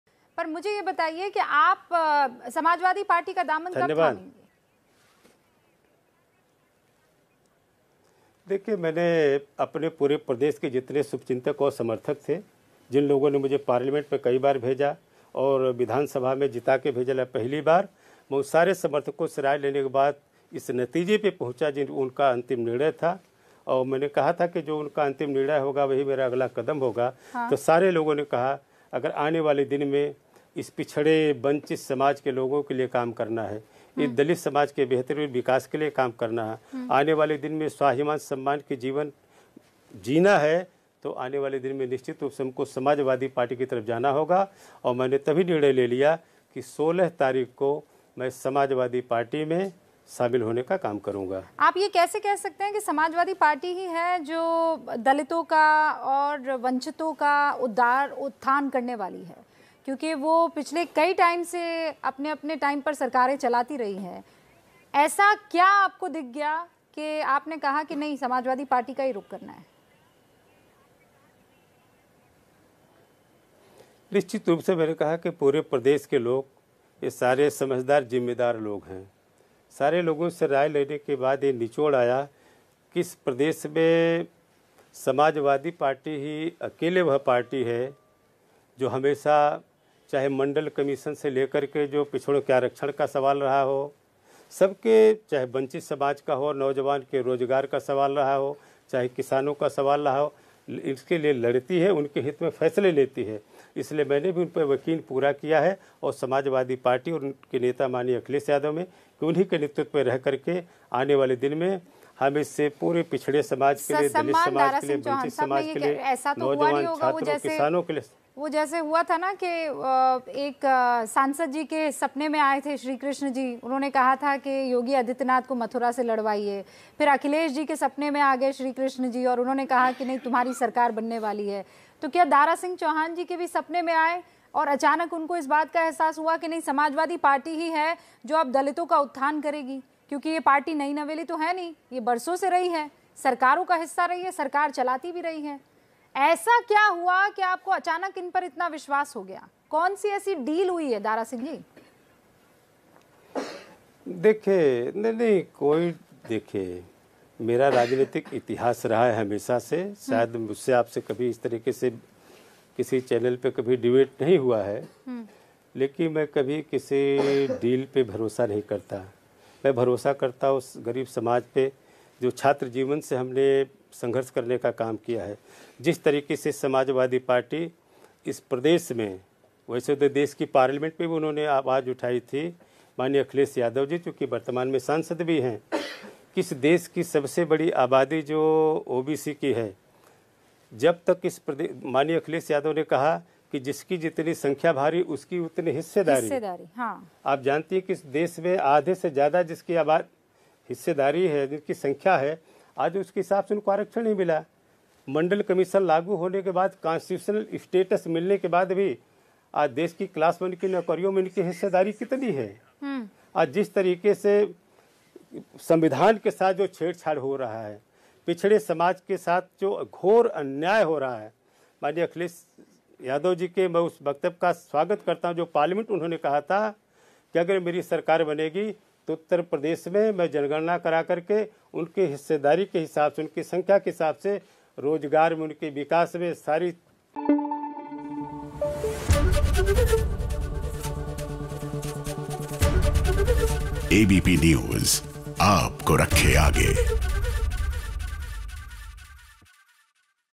Dara Singh Chauhan ने ABP से बताया- इस दिन SP में होंगे शामिल
दारा सिंह चौहान (Dara Singh Chauhan) ने योगी कैबिनेट में मंत्री पद से इस्तीफा दे दिया है. दारा सिंह मऊ जिले की मधुबन विधानसभा सीट से विधायक हैं.